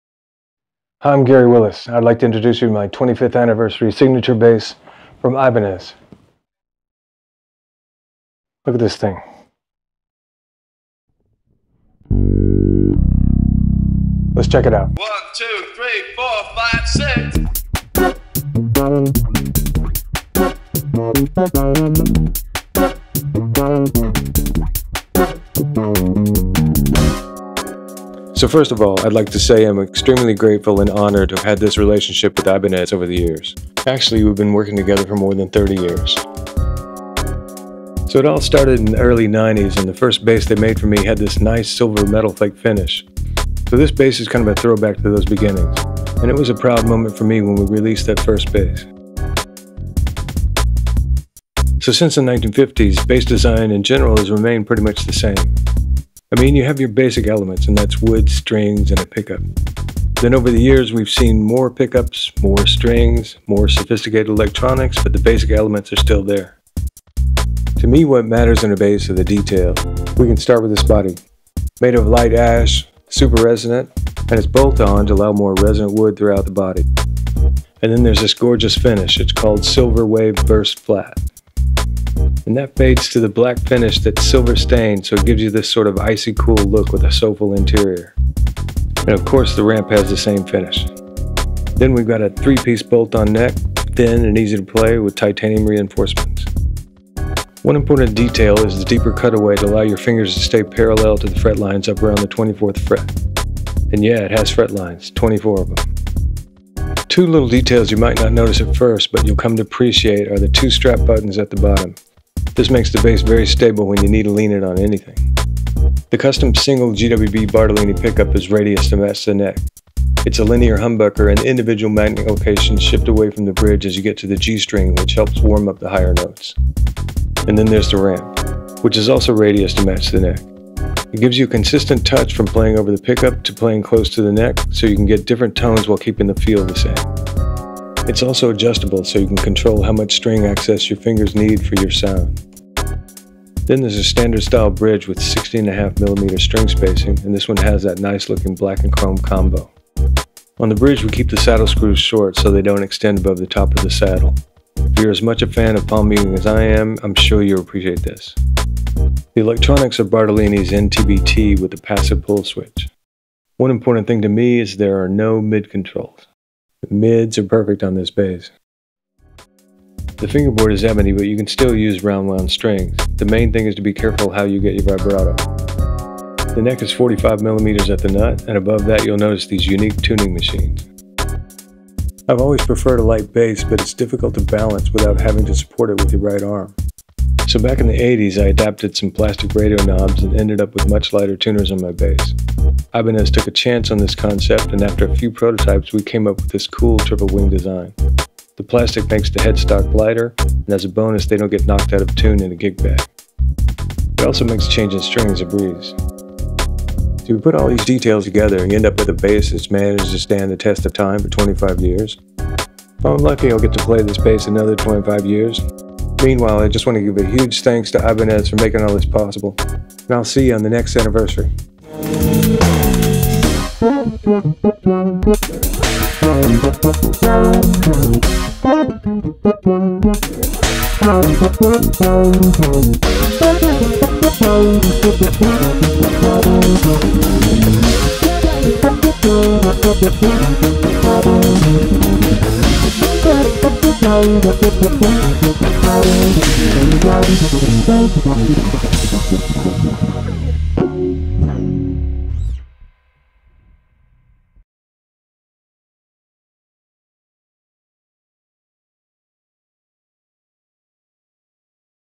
Ibanez GWB 25th Anniversary - Gary Willis Signature Bass
We're thrilled to unveil the newest chapter in the iconic GWB signature series, meticulously designed in collaboration with legendary bassist Gary Willis. This 25th Anniversary edition honors the legacy of the original GWB while spicing up the visuals with a futuristic Silver Wave Burst Flat finish.